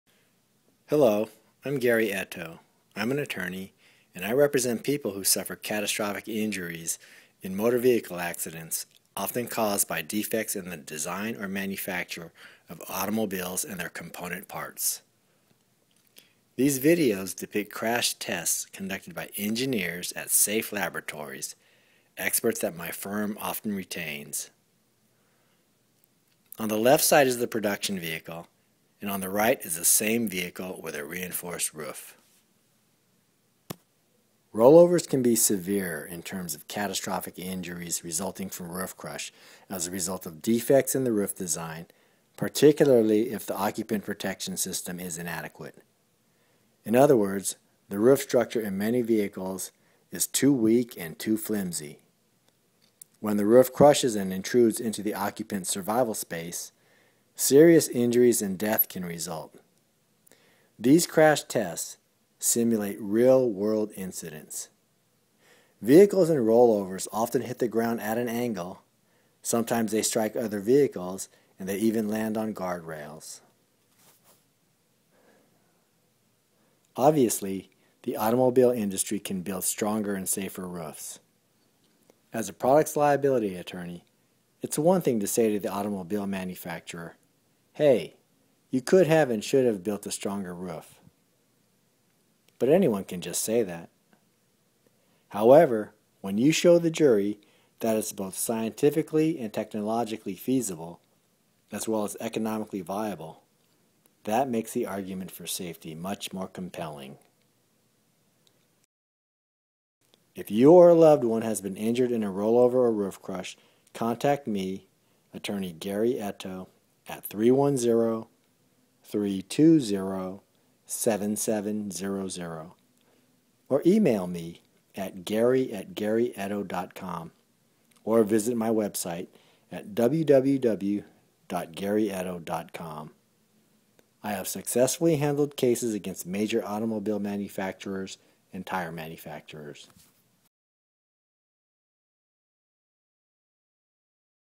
Roof Crush Accidents
Roof Crush Accidents showing the difference between unreinforced and reinforced car roofs

Watch what happens when a car that is certified for  crash safety by NHTSA is dropped from 3 feet above the ground

During an upside down crash or side impact crash the car roof can fail, causing serious injury or death to passengers.

With only $20 of reinforcing material, car roofs could be made much safer than current crash requirements.